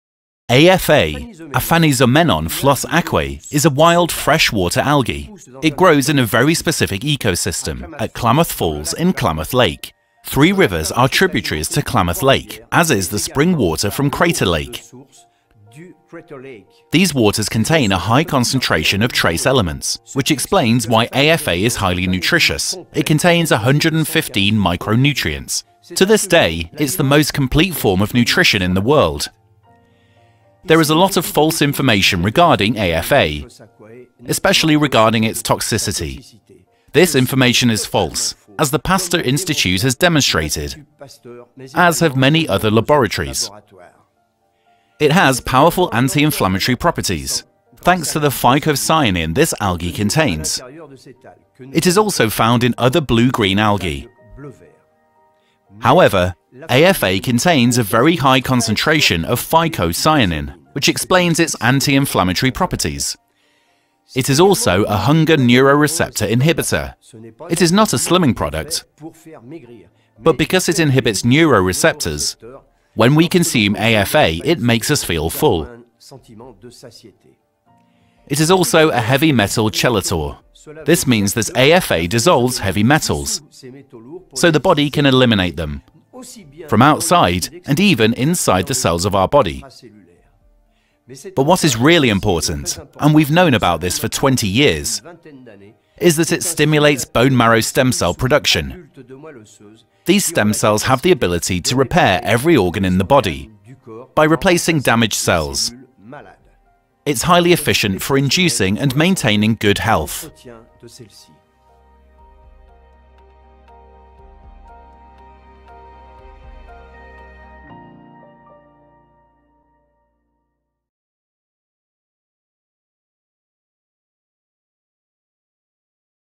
KLAMATH's Blue Green Algae (AFA): THE TRUTH - Jacques PRUNIER SynerJ Health
French biologist researcher Jacques PRUNIER speak about Klamath's blue-green algae (Aphanizomenon Flos Aquae / AFA), AlphaOne, and its amazing properties from which he developed SynerStem and then SynerBoost.

Klamath's Blue-Green Algae (AFA) is known for its health properties since a very long time and is used in dietary supplement since more than 20 years.

It's a high quality compound used in the all-organic products of SynerJ-Health's catalog since the researches made by the biologist researcher Jacques PRUNIER.

Very rich micro-algae (115 micro-nutrients including phycocyanin) and well balanced, Klamath's AFA is a superfood which has the amazing property of enhancing adult bone marrow stem cells production, migration and proliferation.

Contact now your SynerJ-Health's advising agent if you want to benefits from those products or if you have any questions: